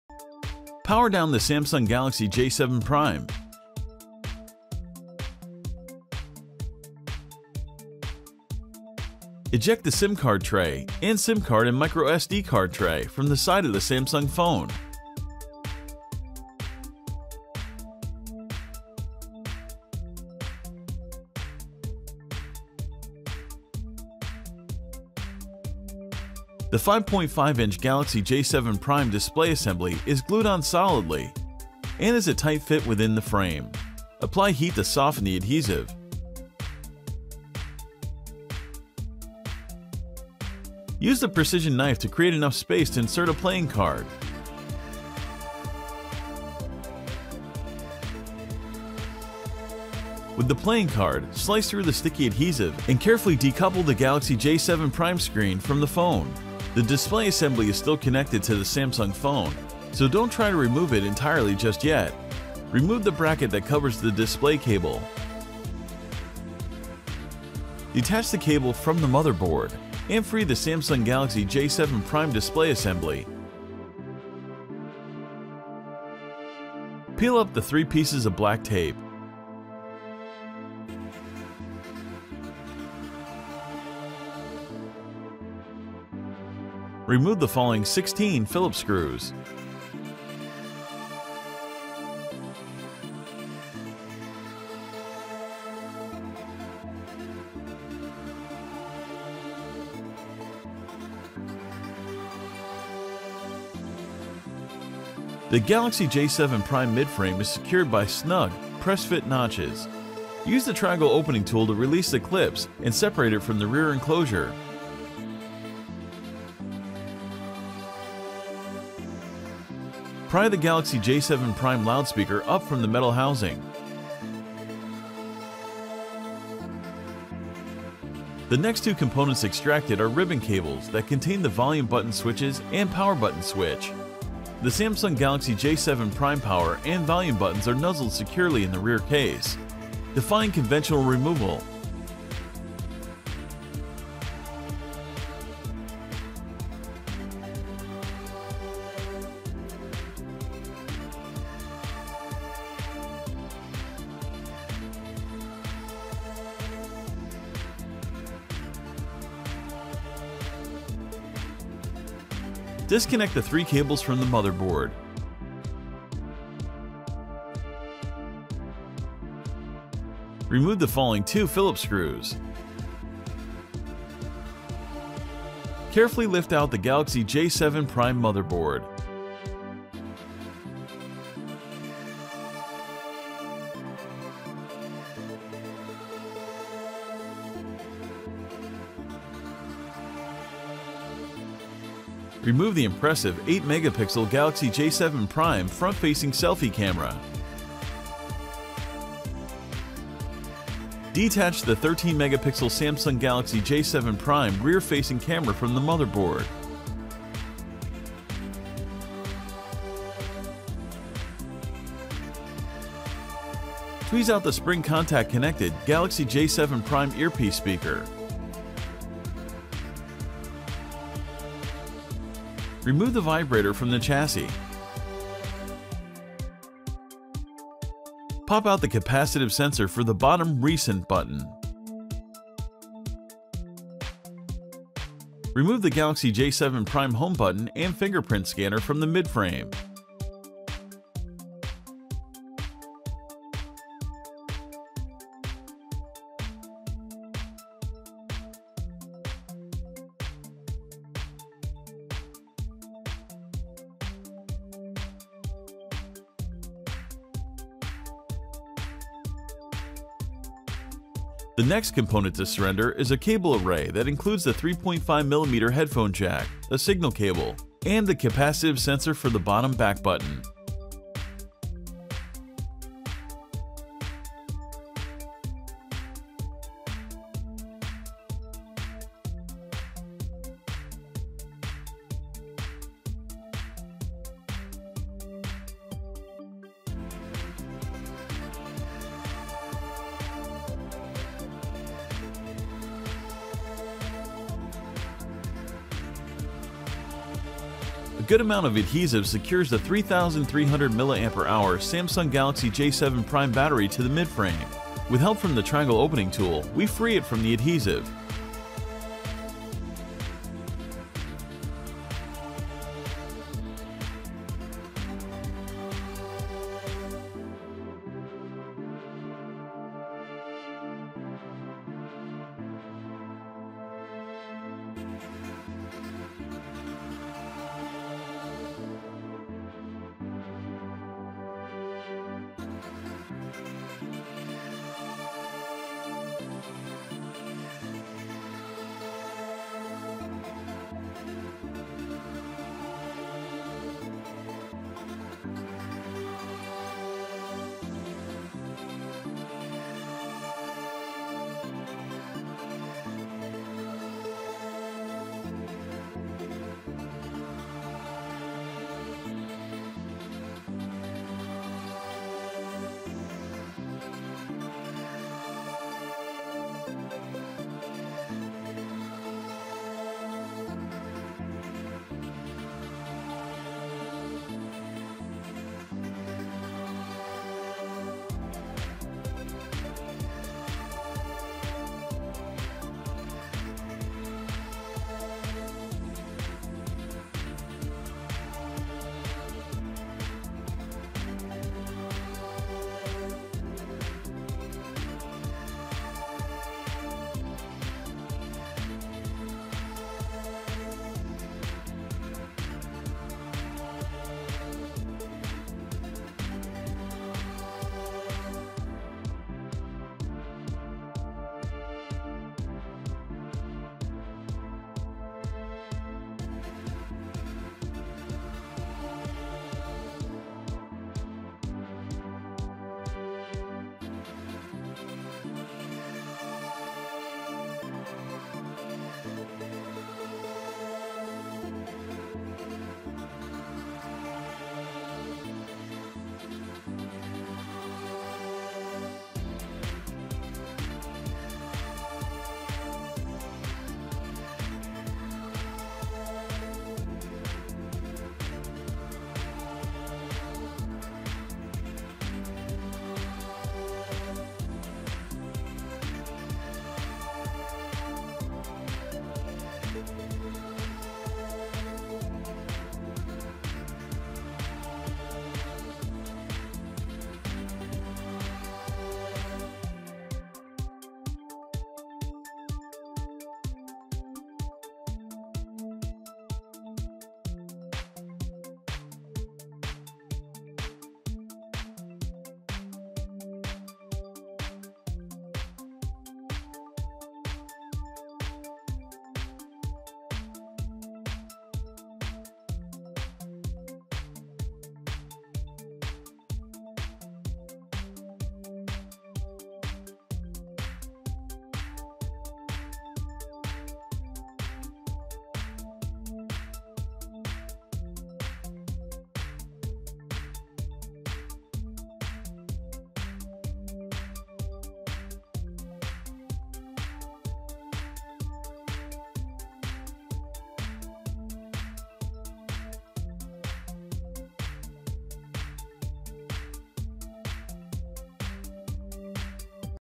Bên trong chiếc GALAXY J7
Samsung Galaxy J7 Thủ Teardown và các bước lắp ráp lại video Hướng dẫn:

bước 1

Power xuống Samsung Galaxy J7 Prime.

Bước 2

Eject khay thẻ SIM, và thẻ SIM và khay thẻ nhớ microSD, từ phía bên của điện thoại Samsung.

Bước 3 (Samsung Galaxy J7 Thủ Display hội)

Các 5,5-inch Galaxy J7 Thủ màn hình lắp ráp được dán trên kiên cố và là một sự phù hợp chặt chẽ trong khung. Áp dụng nhiệt để làm mềm các chất kết dính.

Sử dụng dao chính xác để tạo đủ không gian để chèn một thẻ chơi.

Với lát chơi thẻ qua keo dính và cẩn thận tách màn hình Galaxy J7 Thủ khỏi điện thoại.

Việc lắp ráp màn hình vẫn còn kết nối với điện thoại Samsung, do đó, không cố gắng để loại bỏ nó hoàn toàn chỉ được nêu ra.

Tháo khung bao gồm các cáp màn hình.

Tháo cáp từ bo mạch chủ và giải phóng lắp ráp màn hình Samsung Galaxy J7 Prime.

Bước 4 (Samsung Galaxy J7 Thủ Midframe)

Peel lên 3 miếng băng đen.

Tháo 16 Phillips vít sau.

Galaxy J7 Thủ midframe được bảo đảm bằng, bậc báo chí-vừa khít. Sử dụng công cụ tam giác mở để phát hành các clip và tách nó ra khỏi bao vây phía sau.

Bước 5 (Samsung Galaxy J7 Thủ Loa, nút âm lượng cáp, và nút Power Cable)

cạy Galaxy J7 Thủ tướng Chính loa lên từ vỏ kim loại.

2 thành phần tiếp theo được chiết xuất là dây ruy băng có chứa các nút âm lượng chuyển mạch và nút nguồn switch. Sức mạnh và khối lượng các nút Samsung Galaxy J7 Thủ đang rúc vào một cách an toàn trong trường hợp phía sau, bất chấp loại bỏ thông thường.

Bước 6 (Samsung Galaxy J7 Thủ Bo mạch chủ)

Ngắt kết nối 3 dây cáp từ bo mạch chủ.

Tháo 2 ốc vít Phillips sau.

Cẩn thận nhấc ra bo mạch chủ Galaxy J7 Prime.

Bước 7 (Samsung Galaxy J7 Thủ Front-Facing Camera và phía sau máy ảnh)

Tháo ấn tượng 8-megapixel Galaxy J7 Thủ phía trước “tự sướng” camera.

Tháo 13-megapixel Samsung Galaxy J7 Thủ camera phía sau từ bo mạch chủ.

Bước 8 (Samsung Galaxy J7 Thủ Tai nghe Loa, Vibrator, nút Home với quét vân tay, Headphone Jack, Cable tín hiệu, và điện dung Cảm biến cho Buttons Bottom)

Tweeze ra vào mùa xuân tiếp xúc kết nối Galaxy J7 Thủ loa tai nghe.

Tháo rung từ khung xe.

Bật ra cảm biến điện dung cho nút đáy “gần đây”.

Tháo nút home Galaxy J7 Prime và máy quét dấu vân tay từ midframe.

Các thành phần bên cạnh đầu hàng là một mảng cáp bao gồm jack cắm tai nghe 3.5mm, cáp tín hiệu, và cảm biến điện dung cho nút đáy “trở lại”.

Bước 9 (Samsung Galaxy J7 Thủ Pin)

Một số lượng tốt của chất kết dính chặt những 3.300 milliampere giờ Samsung Galaxy J7 Thủ pin cho midframe. Với sự giúp đỡ từ công cụ tam giác mở chúng tôi giải phóng nó khỏi dính.

* Để lắp ráp lại thiết bị của bạn, hãy làm theo các bước theo thứ tự ngược.